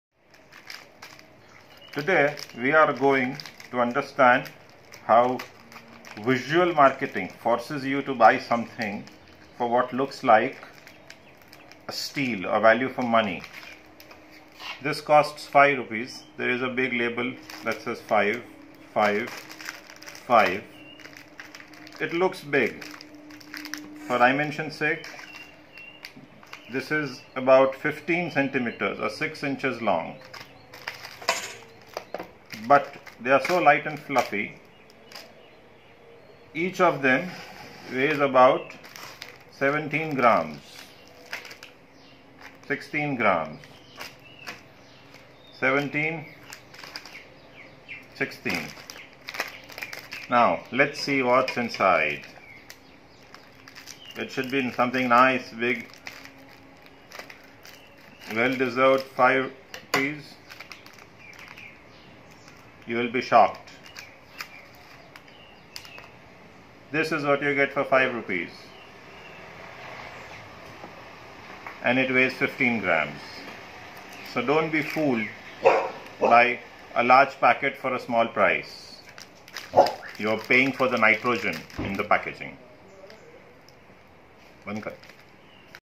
Are you paying for Nitrogen Gas at low price points
I saw this for 5 rupees, and this video is to show you that visual marketing forces the mind into a "value for money" trap that further influences your purchase. The economic definition of a customer is "ability to spend and willingness to buy", both condition to be satisfied. Visual marketing forces you to pay for the nitrogen used in the packaging..